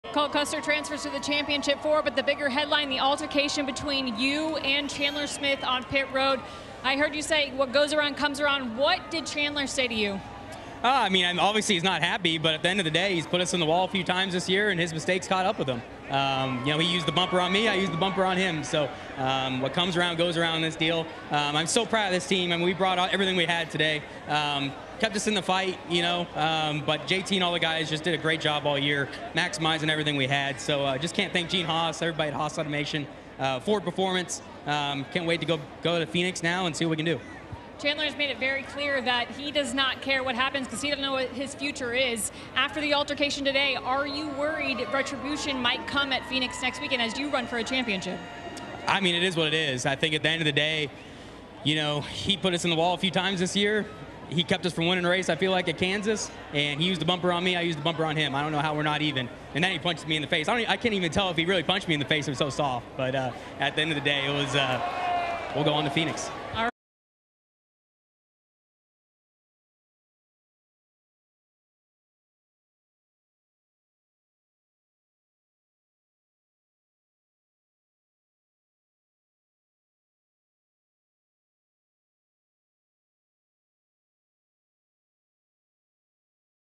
Custer: ‘I don’t know how we’re not even’ after Chandler Smith confrontation
ole Custer describes his side of the story after contact at Martinsville Speedway led to a post-race exchange with Chandler Smith.

Play every weekend!